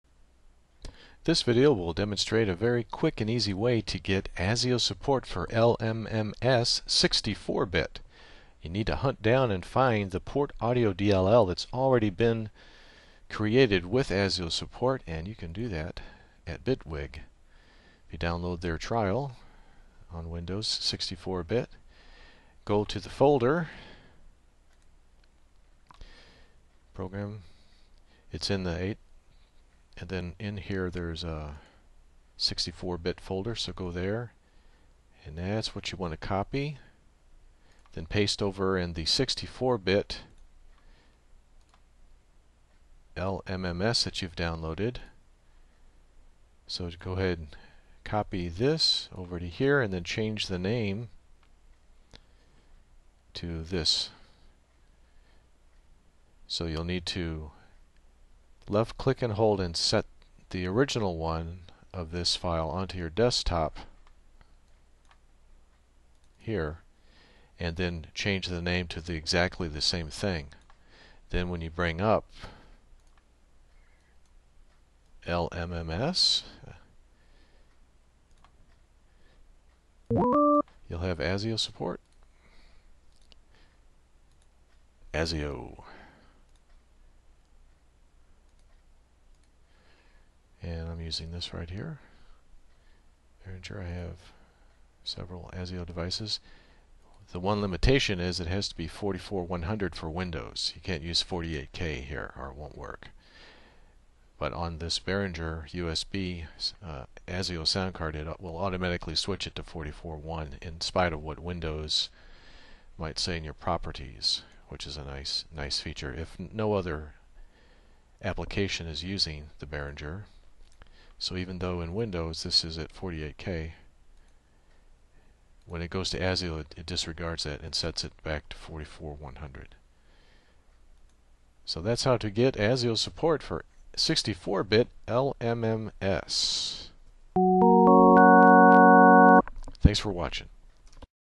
LMMS (64 bit) - ASIO DRIVER SUPPORT - the easy way
This video demonstrates a quick and easy way to get ASIO SUPPORT for LMMS 64 BIT.  It only requires finding a 64 bit portaudio.dll that has already been created with ASIO SUPOORT on an existing product that you can copy and paste over and replace in the LMMS folder where the portaudio-2.dll file resides.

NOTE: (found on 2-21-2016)
HERE ARE BOTH the 64 BIT and 32 BIT portaudio.dll binaries for windows with ASIO support recently compiled in 2015  AND it also has ALL of the windows audio options, MME, WDM, WASAPI, DS
downoad the ZIP file and unzip and open it and you should see
these 2 dll's
libportaudio32bit.dll
libportaudio64bit.dll

MOVE the original portaudio-2.dll from the LMMS folder into aother folder for safe keeping
so that you can replace it back if needed...

go to the GitHub /spatialaudio/ portaudio folder and
rename their libporttaudio32bit.dll/libportaudio64bit.dll  to
libportaudio-2.dll and COPY/PASTE/ it into the native LMMS folder where the original portaudio-2.dll resided.

HERE ARE THE LINKS shown in the video:
1.  Bitwig Studio demo download (the older version that this video is using - newest versions of bitwig do not provide the portaudio.dll)
2.  LMMS 64 bit download

NOTE:  bitwig also has the 32 bit portaudio.dll with ASIO SUPPORT, that you can also use for your 32 bit LMMS.

it is very easy to do this...
1.  just copy/paste the bitwig portaudio-x64.dll into the 64 bit LMMS folder where its own portaudio-2.dll resides.
2.  Then you can left click/hold and move LMMS's own portaudio-2.dll out of the folder and onto the desktop,
3.  then change the name of the remaining  bitwig copy/pasted portaudio-x64.dll to the name that LMMS will accept, which is  portaudio-2.dll.